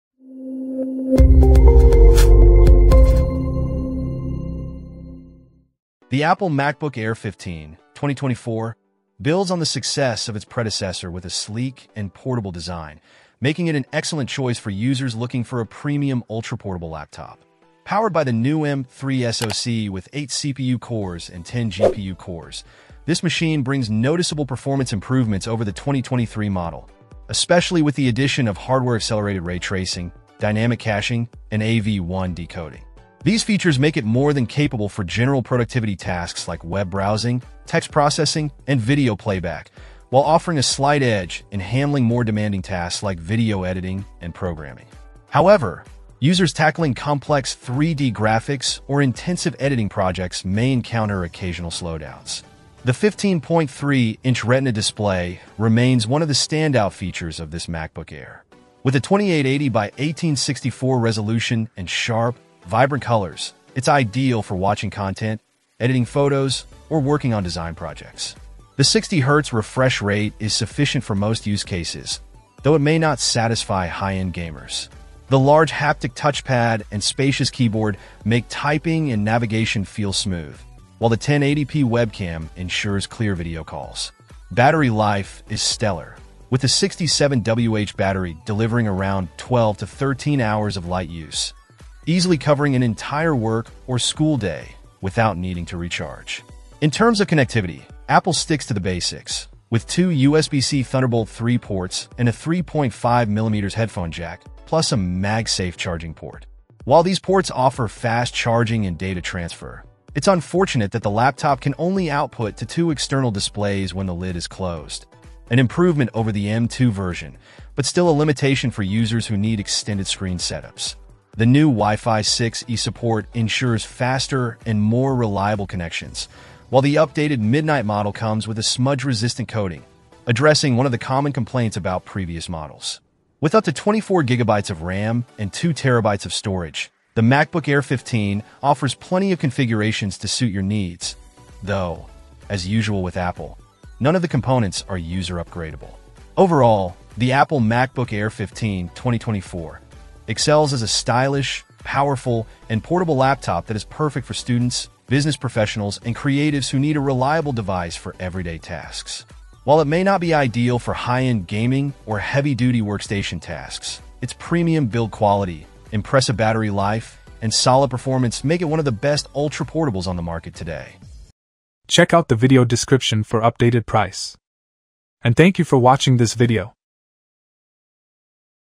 Apple MacBook Air 15 (2024) – The Best Ultra-Thin Laptop for Everyday Use
0:00 Intro
0:07 Review

Things that we mentioned in this video:
Apple 2024 MacBook Air 15-inch : B0CX235DJ6